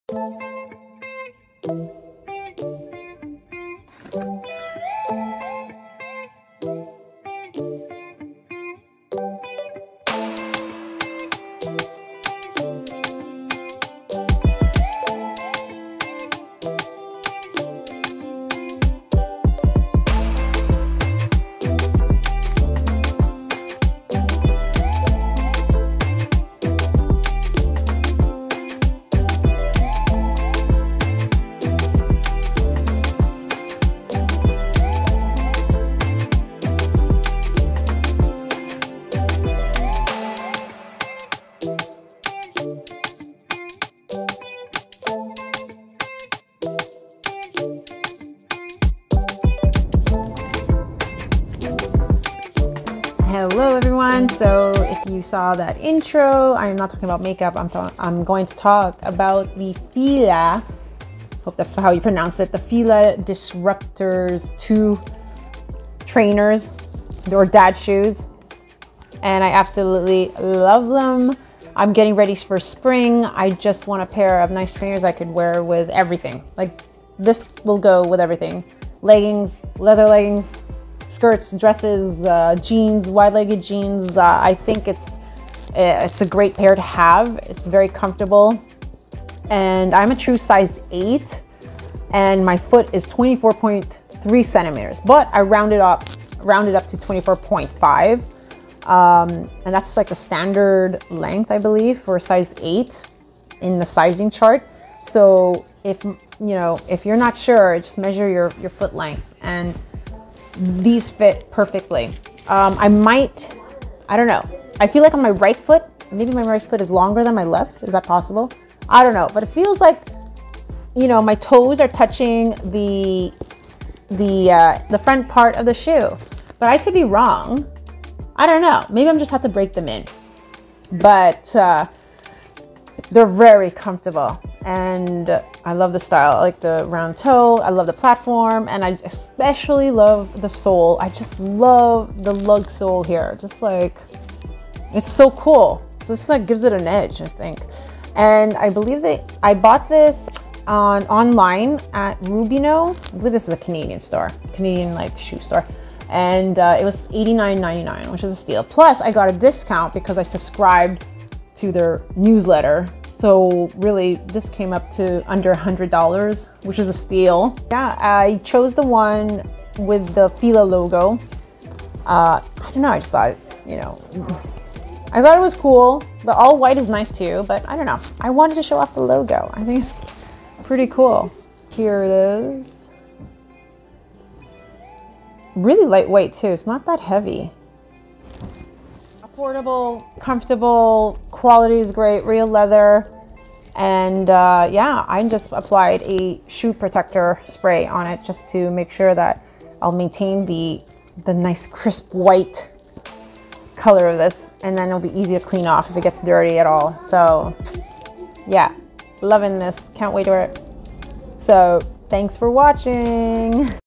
Fila Disruptor II Trainer! A great buy ? March 2, 2022
HI Friends!
I was looking for an affordable  dad shoe/chunky trianer and I found it.. Fila Disruptors II are just cool and comfortable. I wore it with leggings and I see myself wearing them with jeans and dresses. A great shoe buy!

I'm a true size 8 = 24.5 cm foot length